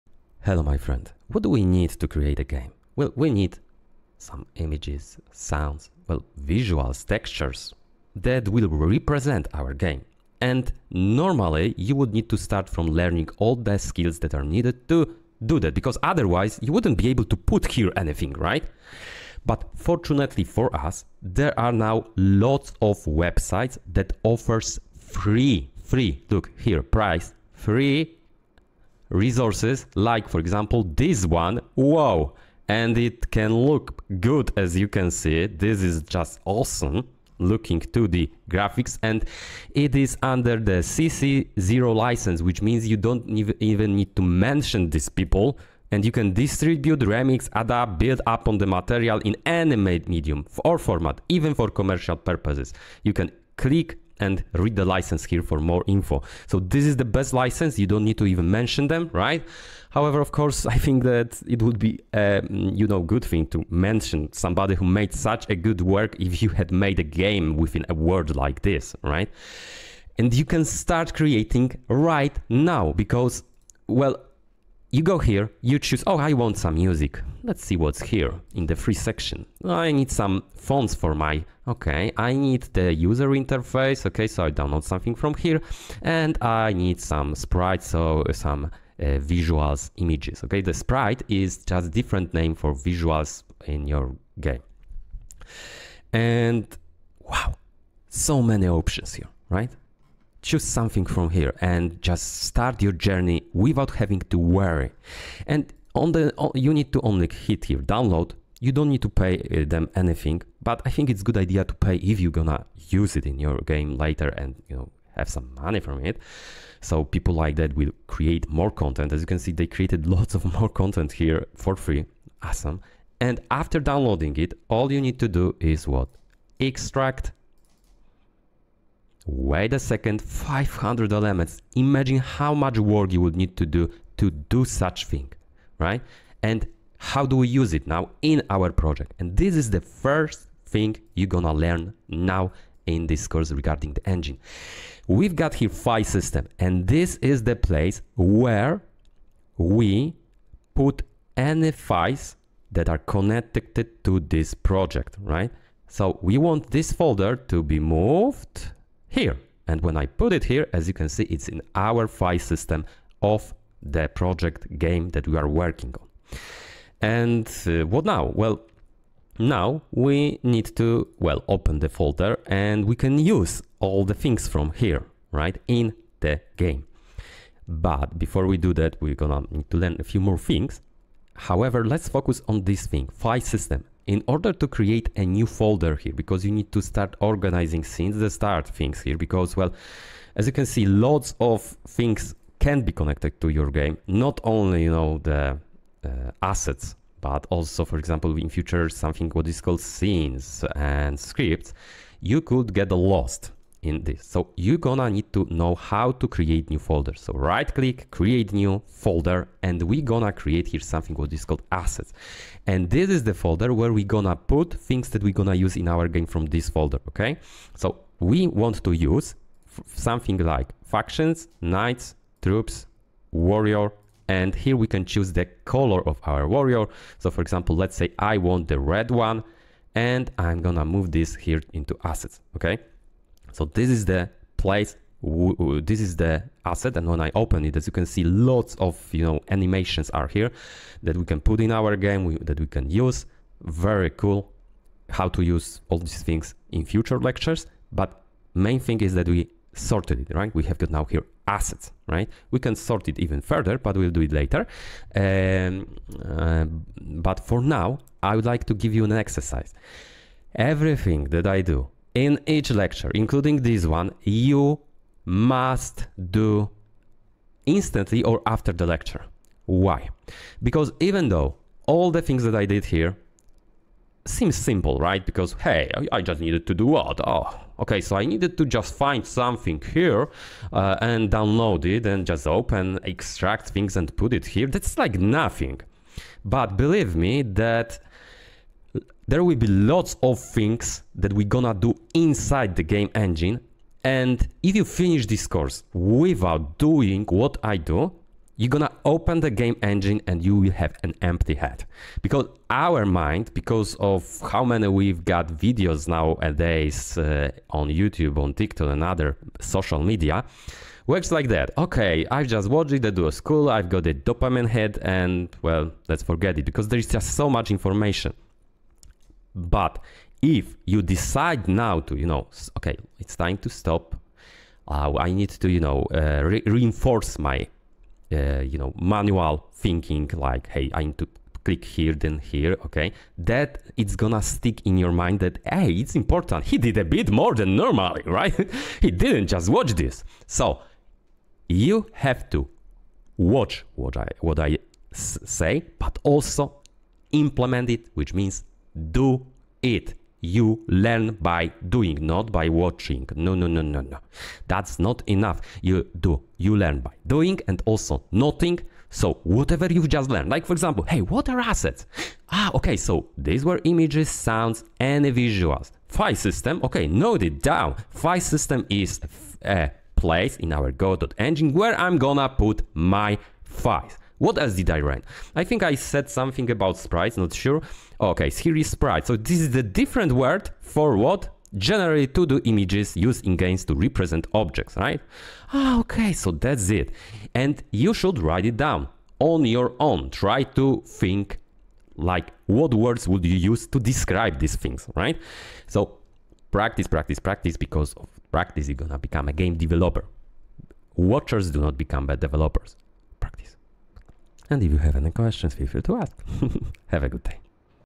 Find & Add ASSETS to Your GAME | Beginner's No-code AI Course (4)
What are assets? Where to download them? How to add them to our project? What is FileSystem?

Welcome back to my Beginner's No-Code AI Game Course! 🚀 In this fourth episode, I'll guide you through finding and adding resources like images and visuals to your game project in Godot. This tutorial is perfect for beginners (noobs) who are just starting their game development journey and want to create games without any programming knowledge, using AI tools.

🔑 Key Topics Covered:
Finding Game Resources - Discover where to find free and paid resources for your game.
Importing Assets into Godot - Step-by-step guide to importing images, visuals, and other resources into your project.
Organizing Your Assets - Best practices for organizing your assets in the Godot project.

00:00:00 - Introduction
00:00:19 - Finding Free Resources
00:01:13 - Exploring the Free Resources
00:02:02 - Downloading and Using the Resources
00:02:16 - Introducing the Game Engine's File System
00:03:01 - Organizing Assets in the File System
00:03:27 - Creating and Using New Folders
00:04:16 - Exercise and Importance of Practice
00:06:44 - Conclusion and Encouragement

💡 Additional Tips:
Use reputable sources for downloading game assets to ensure quality and legality.
Keep your assets organized in folders for easy access and management.
Experiment with AI tools to generate unique visuals for your game.